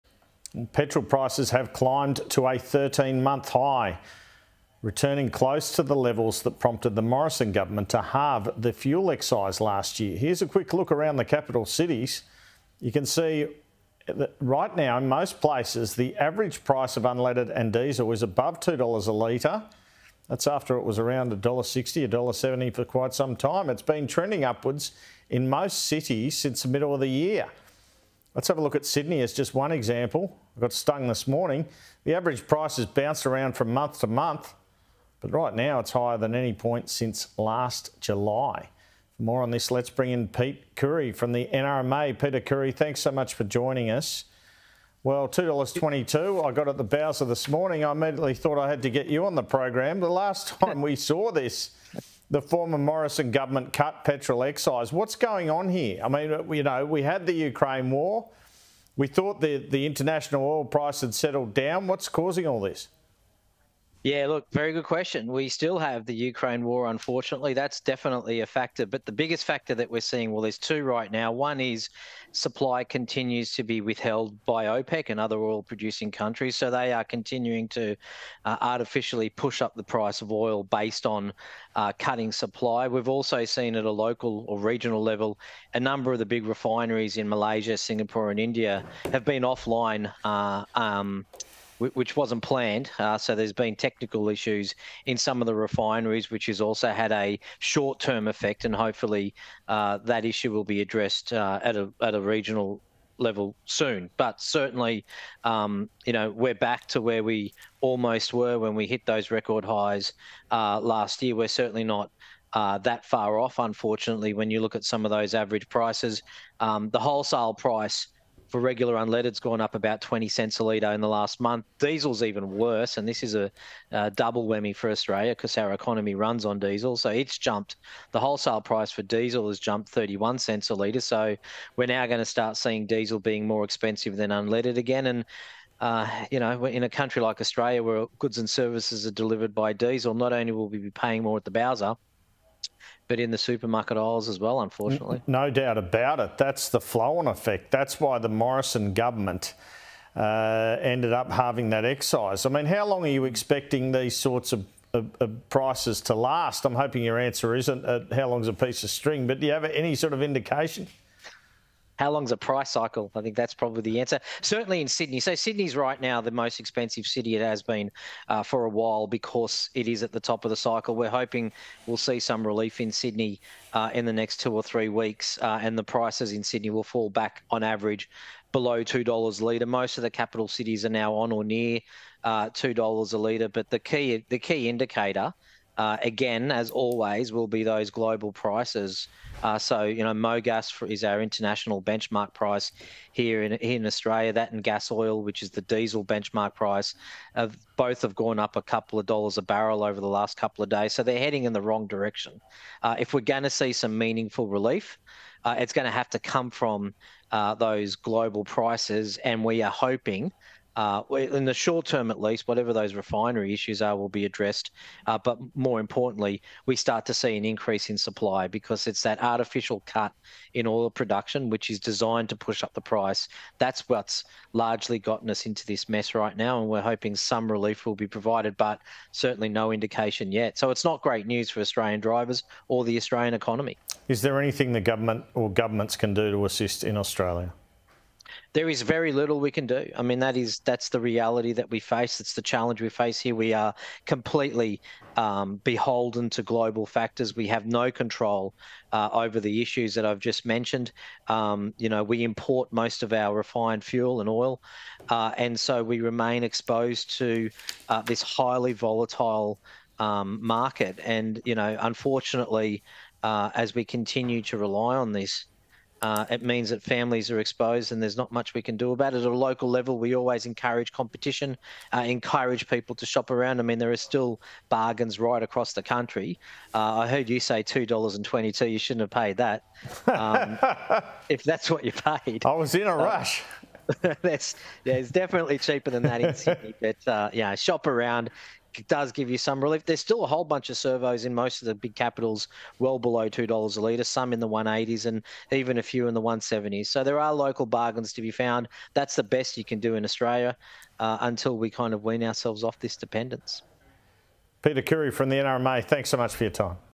Australia remains ‘exposed’ to highly volatile oil market
NRMA Spokesperson Peter Khoury says Australia remains “exposed” to the highly volatile oil market.

“We are completely beholden to global factors,” he told Sky News Australia.